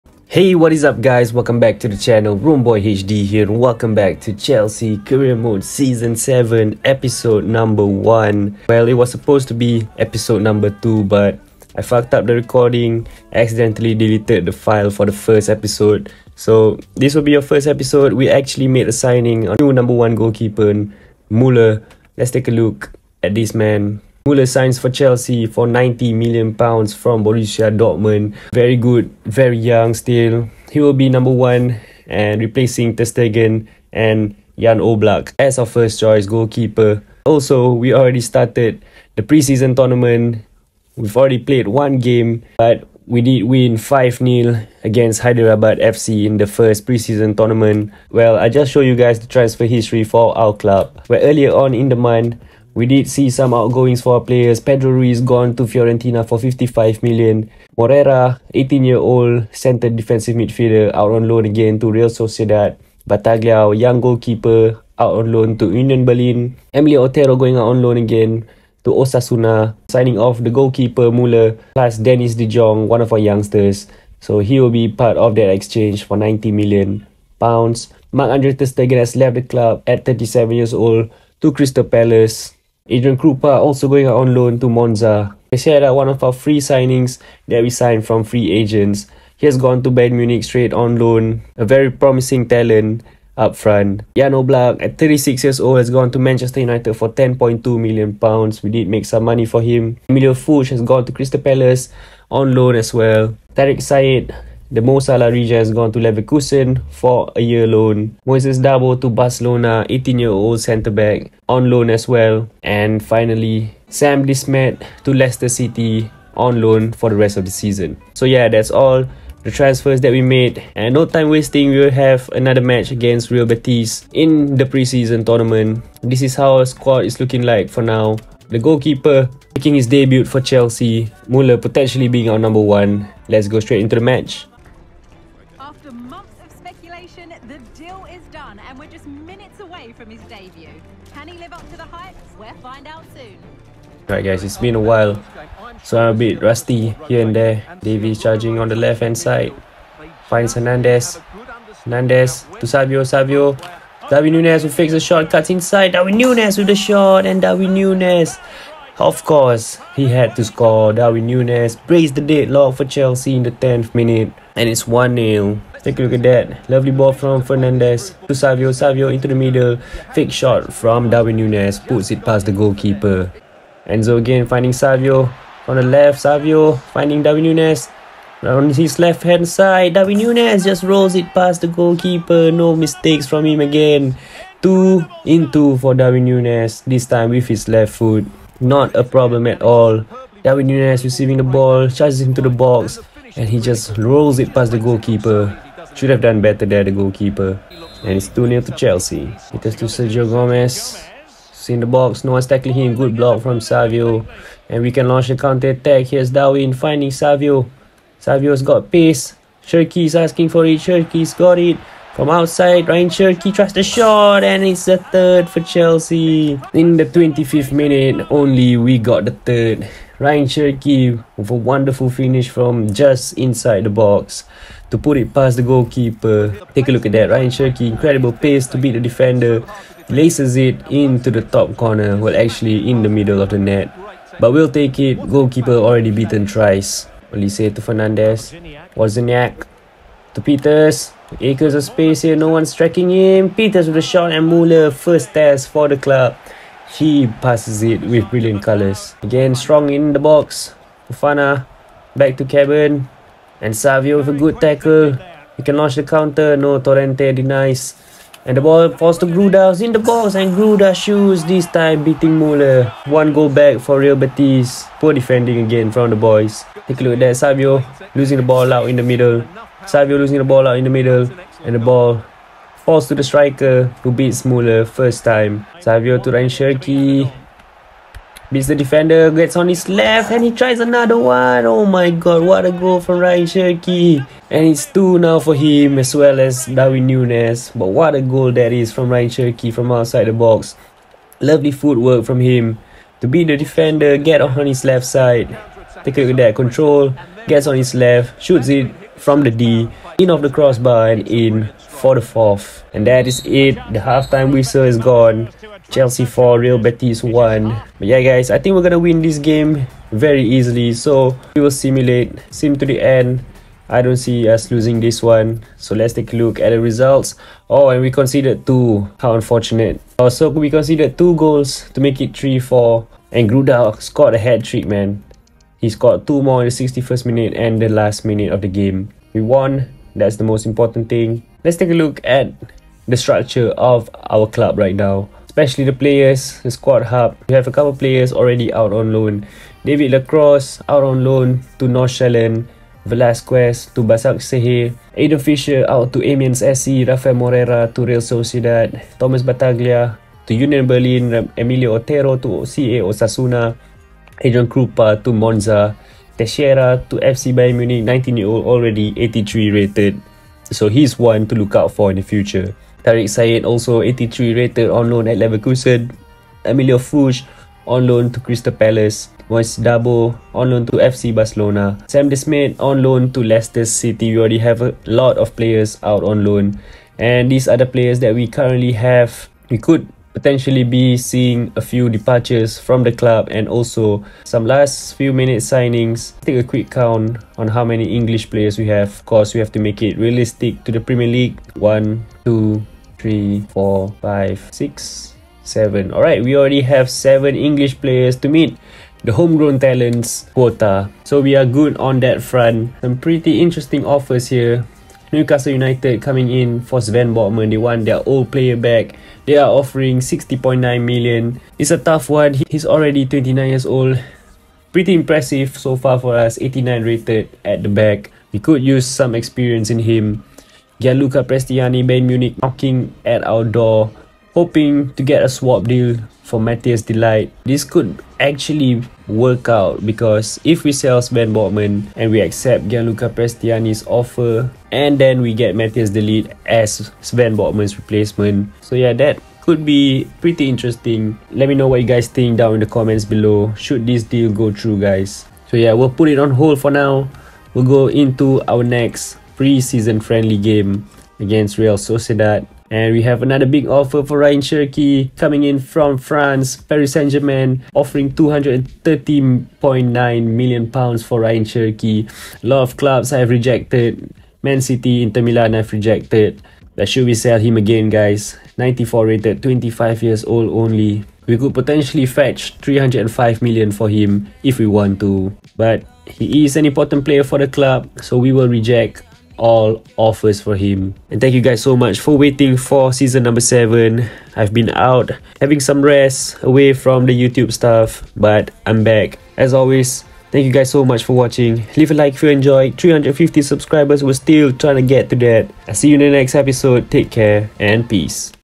Chelsea Career Mode S7E1 - WE ARE BACK!!
WE ARE BACK BABY!! Its season number 7 of this career mode and we are aiming for bigger things this season. Especially that one accolade we havent got yet...